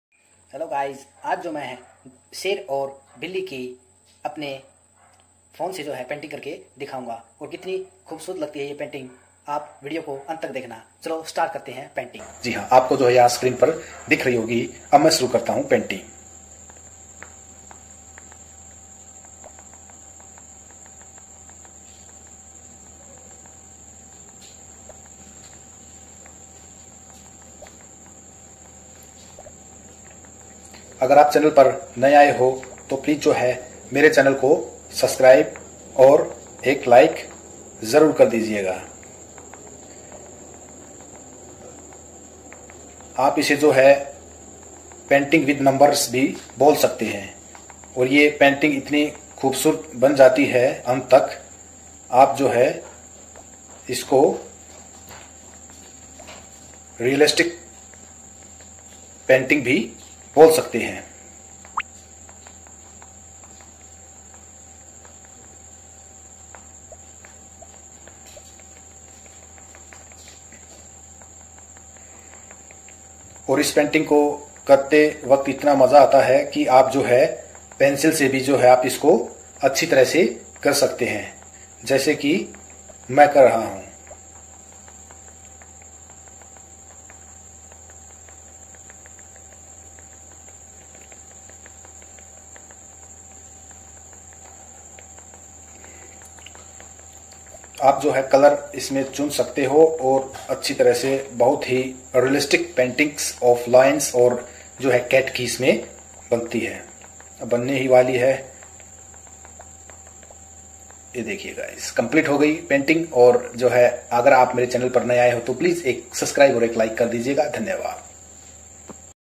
Lion and cat painting by Android phone|lion and cat realistic painting|lion and cat number painting
in this video we painting of lion and cat by my phone android mobile phone
first of all we pin the number on lion and cat sketch
after that with the help of pencil we paint the lion and cat
after painting it is looking like the realistic painting